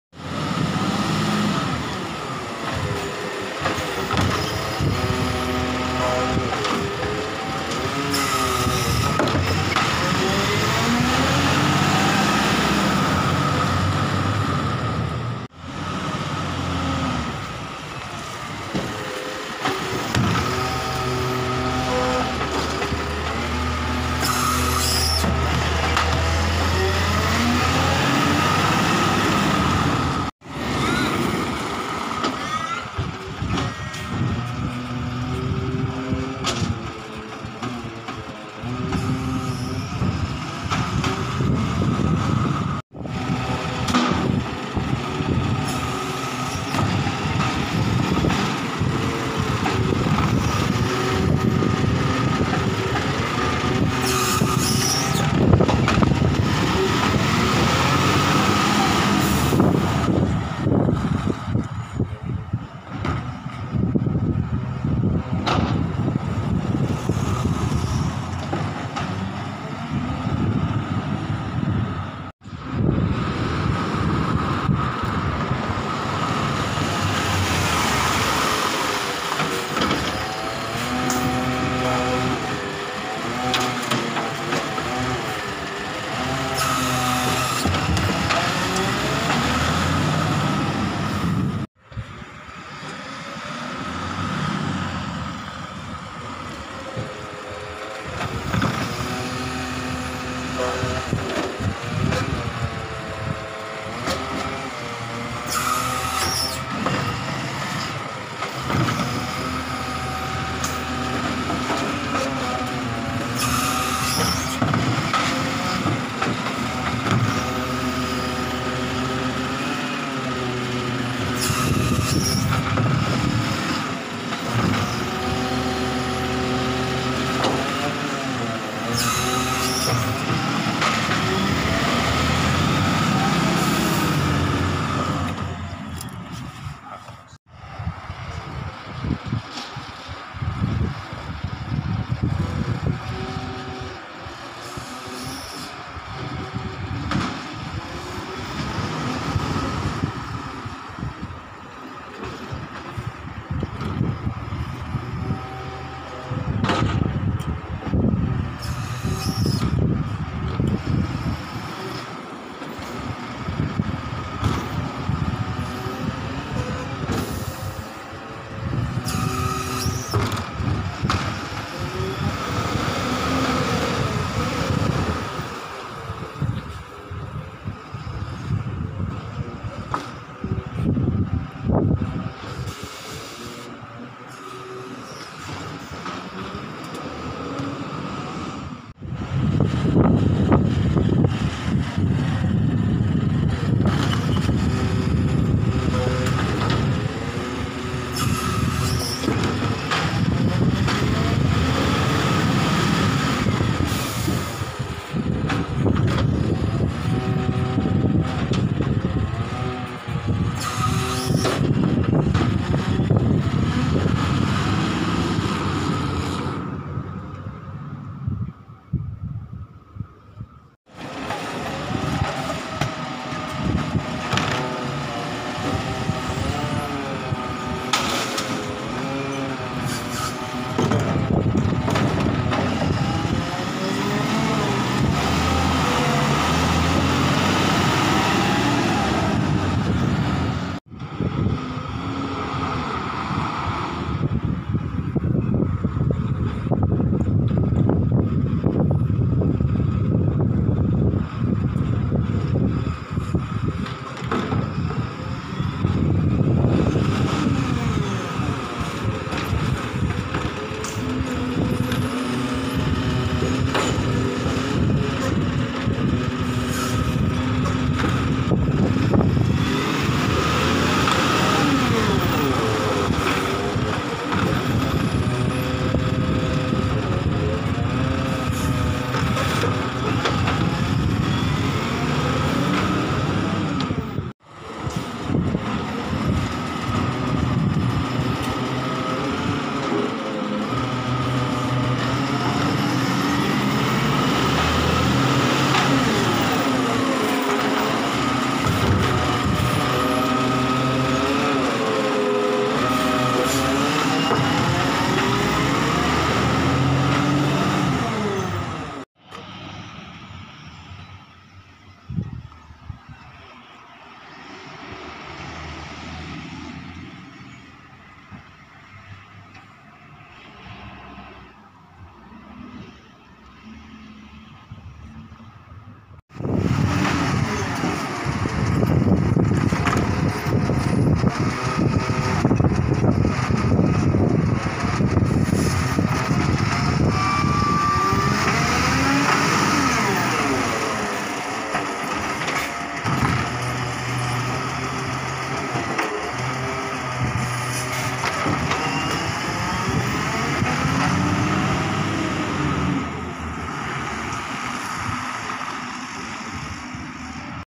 City of Wanneroo garbage chop out
I filmed this chop out with Harrison @WannerooGarbageTrucks thxs to the drivers for letting us film